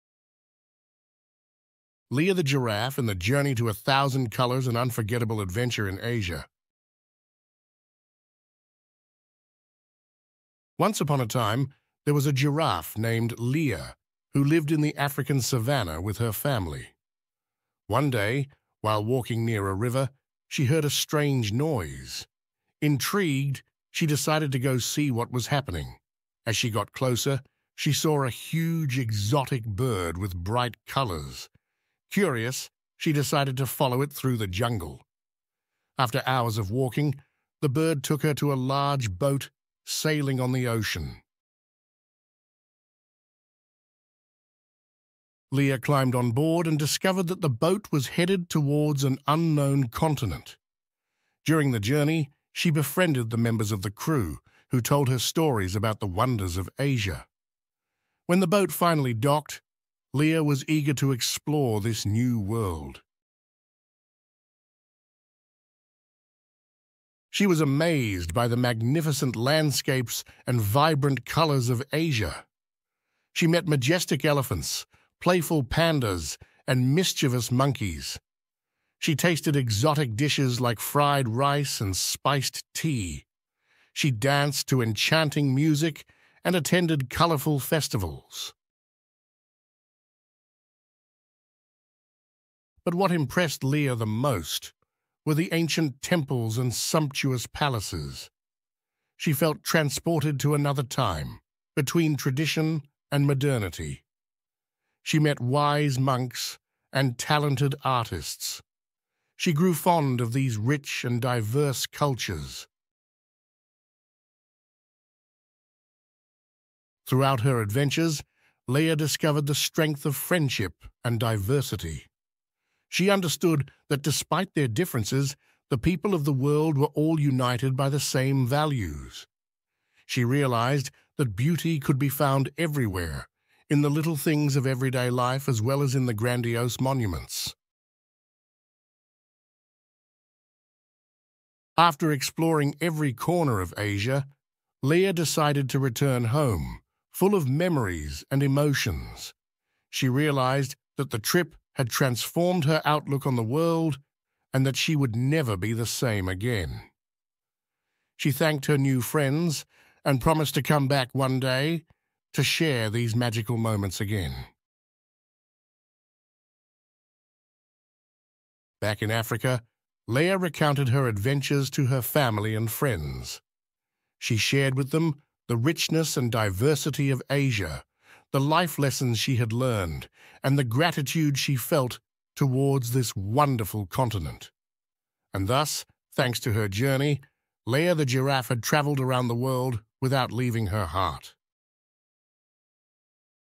Lea the giraffe and the journey in Asia | Fairy Tales For Children
📖 Lea the giraffe embarks on an adventure in Asia after following an exotic bird to a boat, discovering new cultures, friendships, and an enriched worldview before returning home, transformed by her journey.